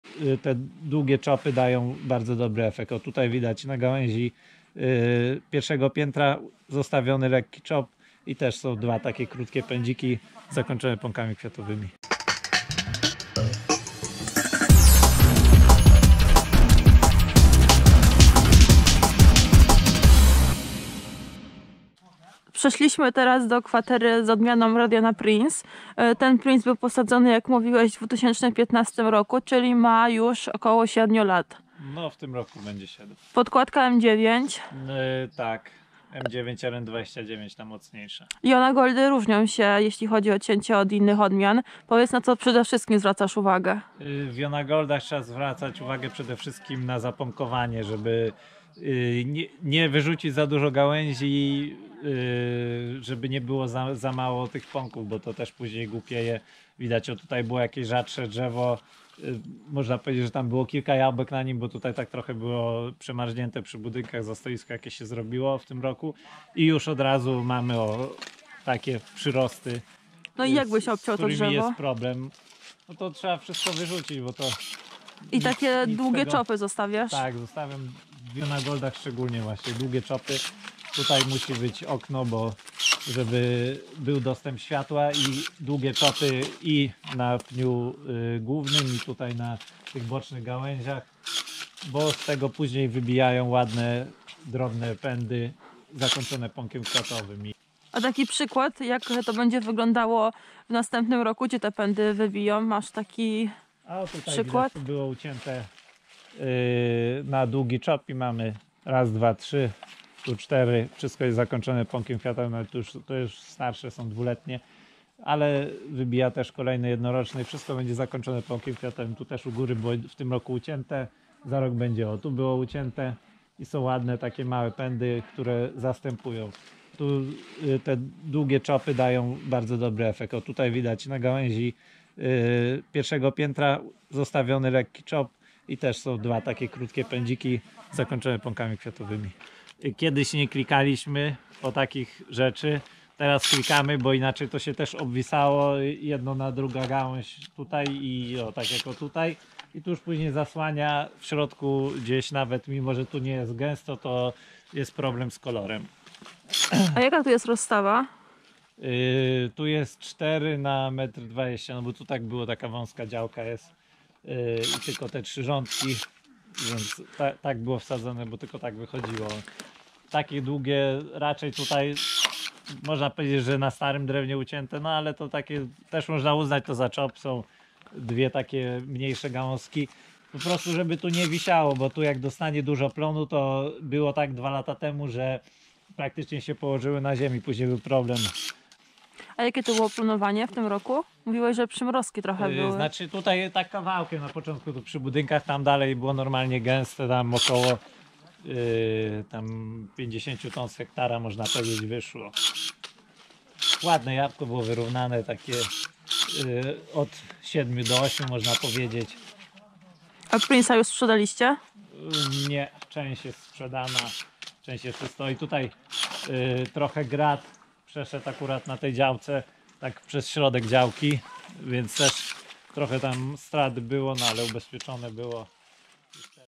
Rozmowy przy cięciu jabłoni 2021 - u Macieja Cybulaka, cz.2
Dziś publikujemy drugą część Rozmów przy cięciu jabłoni nagraną w sadzie Macieja Cybulaka w gminie Łaziska (woj. lubelskie), tym razem na kwaterze odmiany Red Jonaprince.